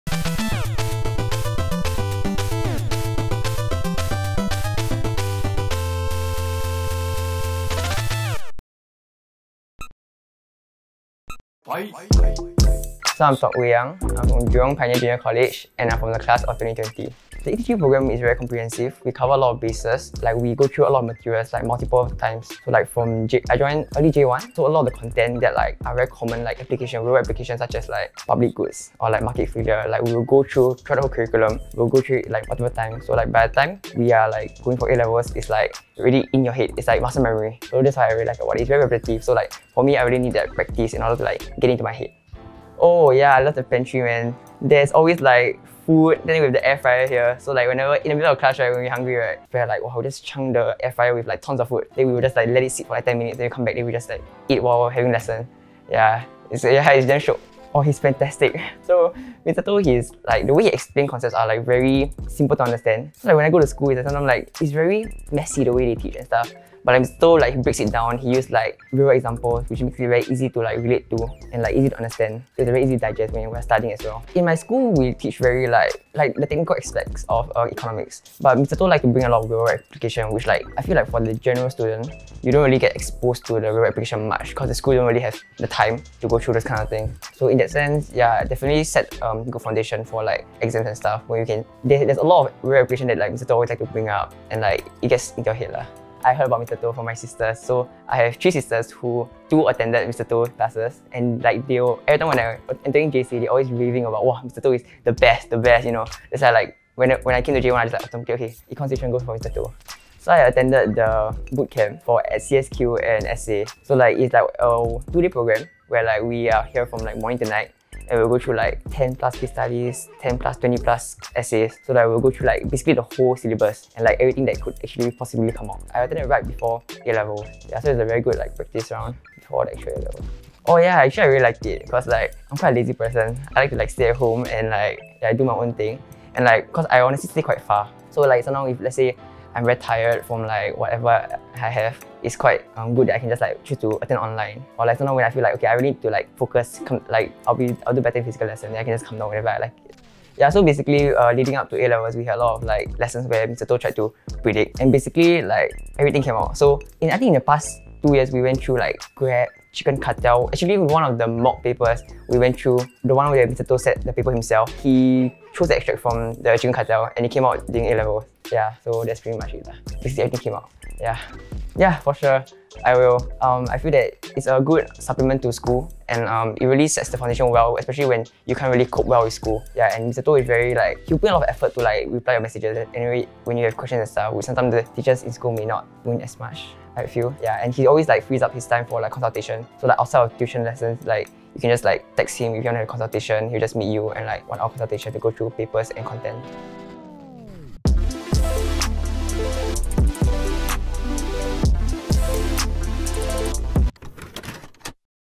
ETG Student Testimonials from Class of 2020 - Tok Wei Yang
Hear what our students have to say about our A Level Economics Tuition Programme at ETG.